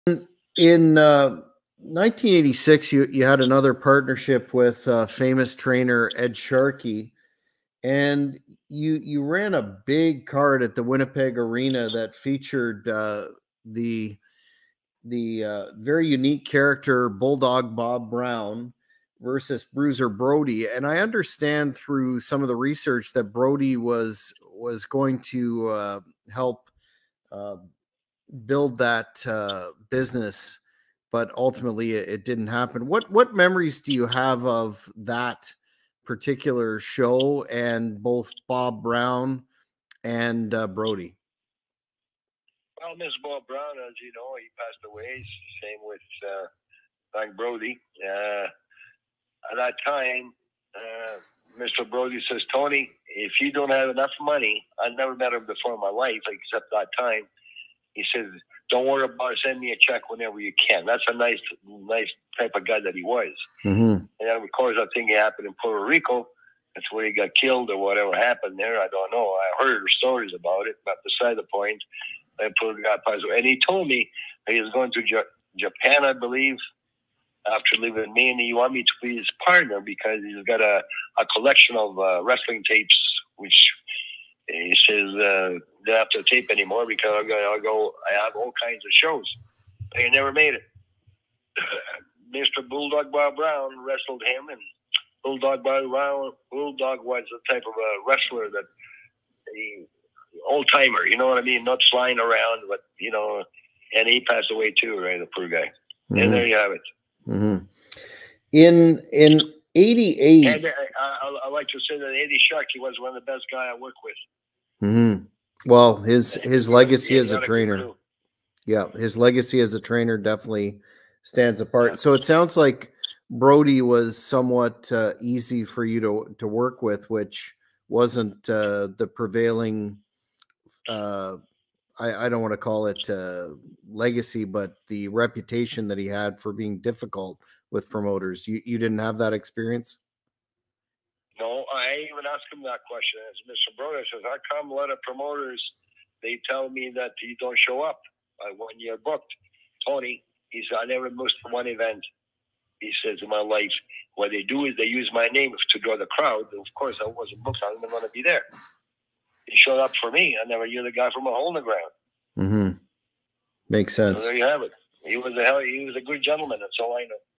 Tony Condello Reveals What Happened When Bruiser Brody Wrestled in Winnipeg
Tony Condello on Bruiser Brody Wrestling in Winnipeg

2nd YouTube Channel @ The Hannibal TV FIGHT & UFO VIDEOS